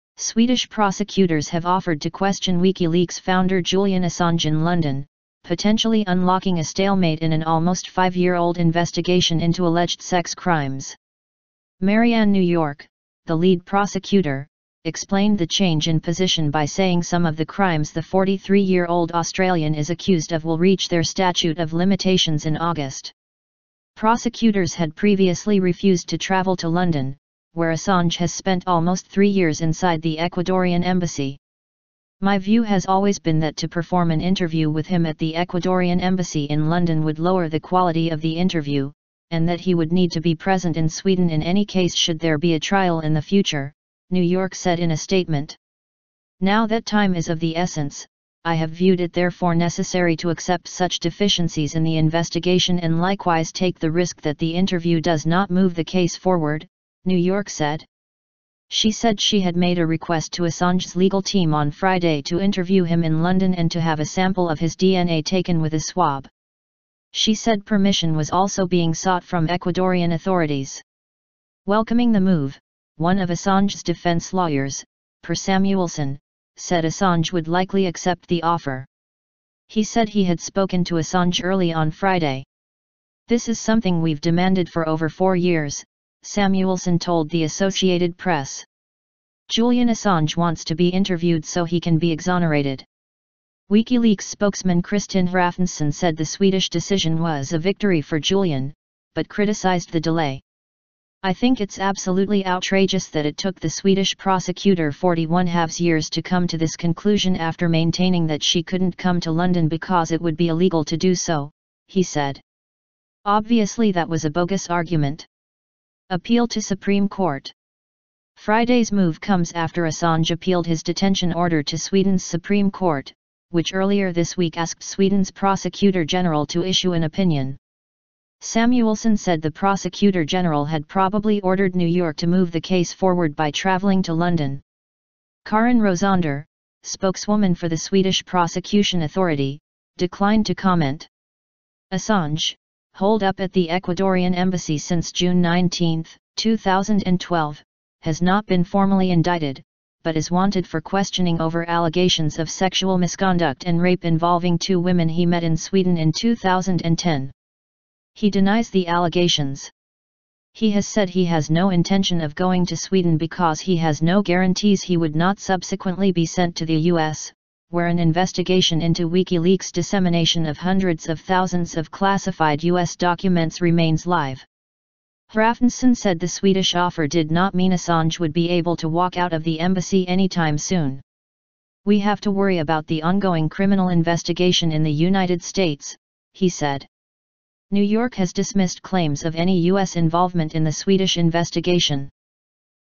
Swedish prosecutors offer to question Assange in London
Swedish prosecutors have offered to question WikiLeaks founder Julian Assange in London, potentially unlocking a stalemate in an almost five-year-old investigation into alleged sex crimes.

Marianne Ny, the lead prosecutor, explained the change in position by saying some of the crimes the 43-year-old Australian is accused of will reach their statute of limitations in August.

Prosecutors had previously refused to travel to London, where Assange has spent almost three years inside the Ecuadorean embassy.

"My view has always been that to perform an interview with him at the Ecuadorean embassy in London would lower the quality of the interview, and that he would need to be present in Sweden in any case should there be a trial in the future," Ny said in a statement.

"Now that time is of the essence, I have viewed it therefore necessary to accept such deficiencies in the investigation and likewise take the risk that the interview does not move the case forward," Ny said.
She said she had made a request to Assange's legal team on Friday to interview him in London and to have a sample of his DNA taken with a swab. She said permission was also being sought from Ecuadorean authorities.

Welcoming the move, one of Assange's defence lawyers, Per Samuelson, said Assange would likely accept the offer. He said he had spoken to Assange early on Friday.

"This is something we've demanded for over four years," Samuelson told The Associated Press. "Julian Assange wants to be interviewed so he can be exonerated."

WikiLeaks spokesman Kristinn Hrafnsson said the Swedish decision was "a victory for Julian," but criticised the delay.

"I think it's absolutely outrageous that it took the Swedish prosecutor 41/2 years to come to this conclusion after maintaining that she couldn't come to London because it would be illegal to do so," he said. "Obviously that was a bogus argument."

Appeal to Supreme Court

Friday's move comes after Assange appealed his detention order to Sweden's Supreme Court, which earlier this week asked Sweden's prosecutor-general to issue an opinion.

Samuelson said the prosecutor-general had probably ordered Ny to move the case forward by travelling to London. Karin Rosander, spokeswoman for the Swedish Prosecution authority, declined to comment.

Assange - holed up at the Ecuadorean embassy since June 19, 2012 - has not been formally indicted, but is wanted for questioning over allegations of sexual misconduct and rape involving two women he met in Sweden in 2010. He denies the allegations.

He has said he has no intention of going to Sweden because he has no guarantees he would not subsequently be sent to the US., where an investigation into WikiLeaks' dissemination of hundreds of thousands of classified US documents remains live.

Hrafnsson said the Swedish offer did not mean Assange would be able to walk out of the embassy anytime soon.

"We have to worry about the ongoing criminal investigation in the United States," he said.

Ny has dismissed claims of any US involvement in the Swedish investigation.